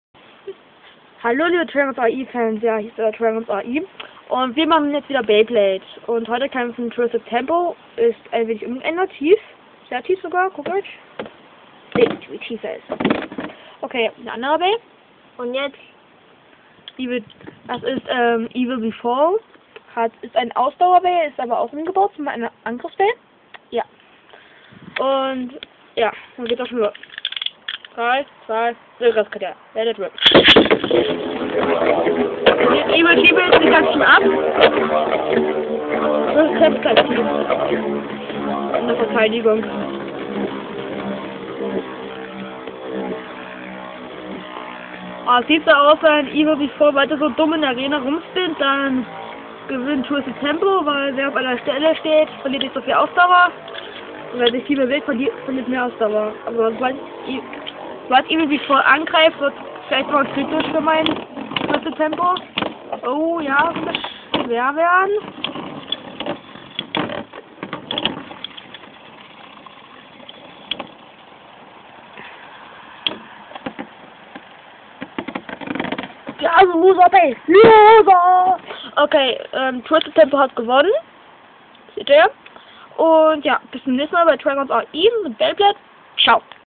1. Turnier von Beyblade Twisted Tempo VS. Evil Befall
Hier bei dem Match fängt unser erstes Turnier von Beyblade an.
Das erste Turniermatch die Kämpfer sind Twisted Tempo und Evil Befall.